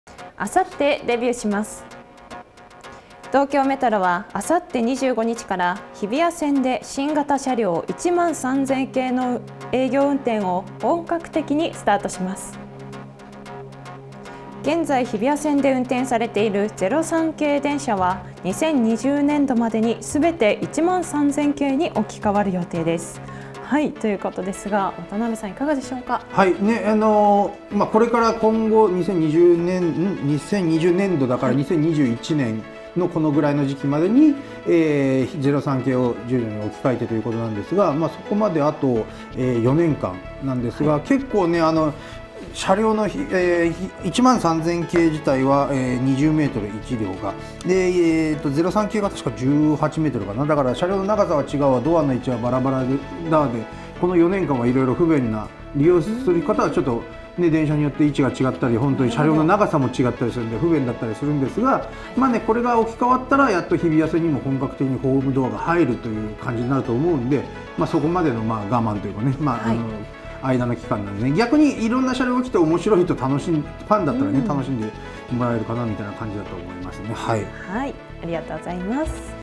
日比谷線13000系デビュー【鉄道ニュース546】
東京メトロは、あさって２５日から日比谷線で新型車両１３０００系の営業運転を本格的にスタートします。
現在、日比谷線で運転されている０３系電車は２０２０年度までに、すべて１３０００系に置き換わる予定です。
コメンテータ：渡辺雅史さん（ライター）
キャスター：近藤智美（鉄道チャンネル）
鉄道ニュースアプリ